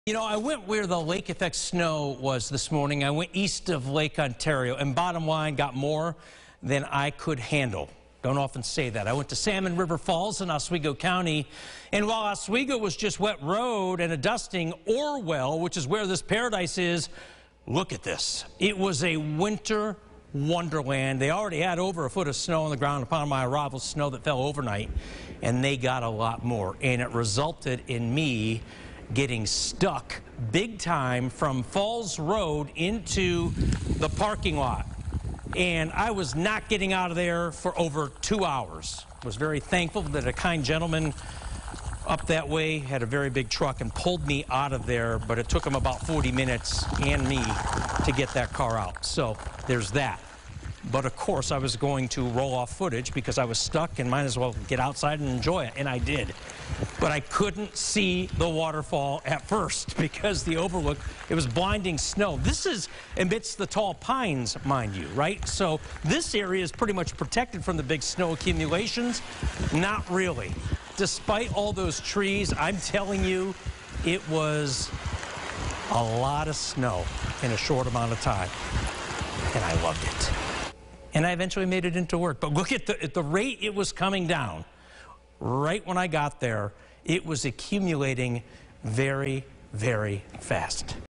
Kucko's Camera: Stuck in the snow at Salmon River Falls
Today John Kucko took his camera east of Lake Ontario to Salmon River Falls in Orwell, where snow trapped him in his car for more than two hours.